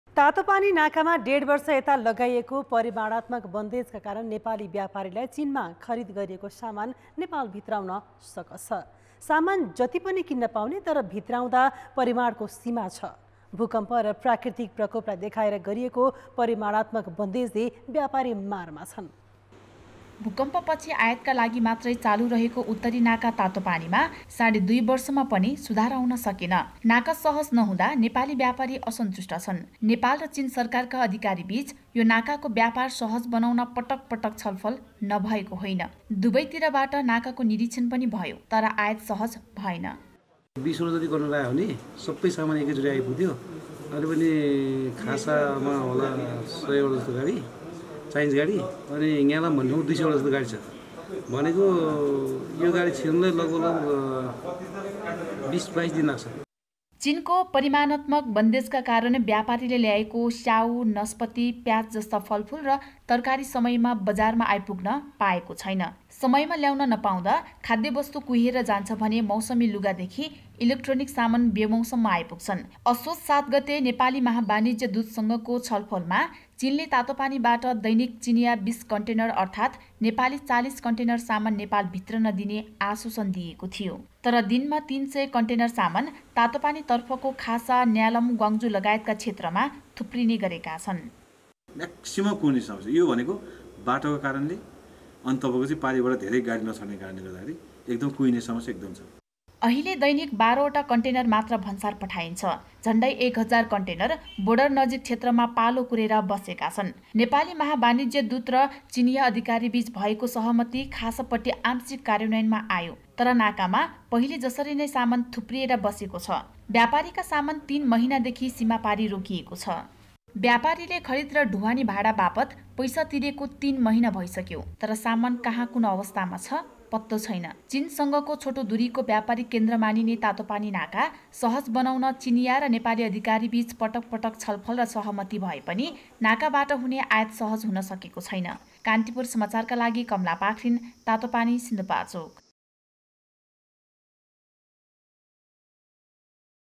डेढ वर्षदेखि तातोपानी नाकाबाट सामान भित्र्याउन सास्ती
तातोपानी नाकामा डेढवर्ष यता लगाइएको परिमाणात्मक बन्देजका कारण नेपाली व्यापारीलाई चीनमा खरिद गरिएको सामान नेपाल भित्र्याउन सकस छ । सामान जति पनि किन्न पाउने तर भित्र्याउँदा परिमाणको सीमा छ । भूकम्प र प्राकृतिक प्रकोपलाई देखाएर गरिएको परिमाणात्मक बन्देजले व्यापारी मारमा छन् ।